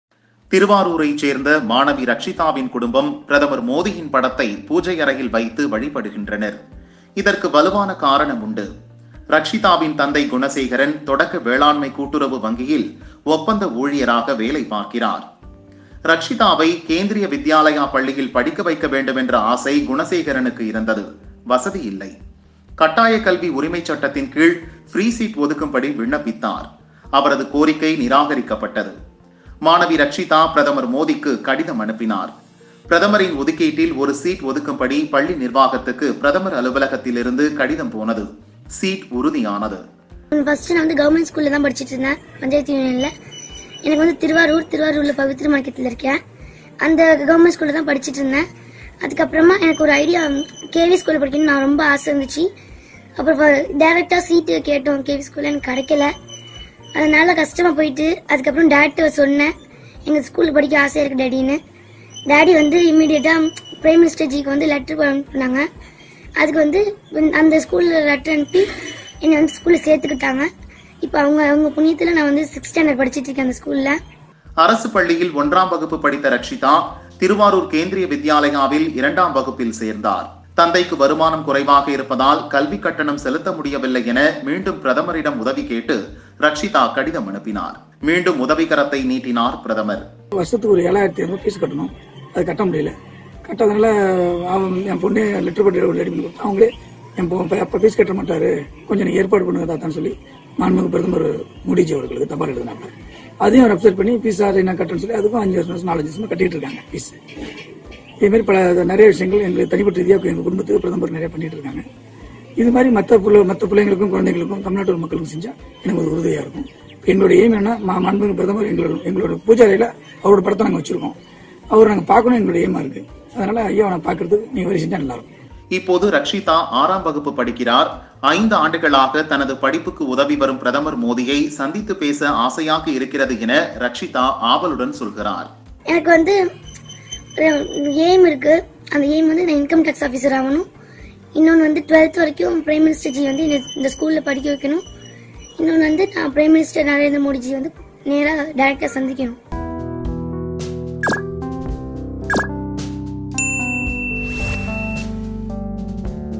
பிரதமரை சந்திக்க ஆசை: மாணவி ரக்ஷிதா
திருவாரூரைச் சேர்ந்த மாணவி
ரக்க்ஷிதாவின் குடும்பம்
பிரதமர் மோடியின் படத்தை
பூஜை அறையில் வைத்து வழிபடுகின்றனர்.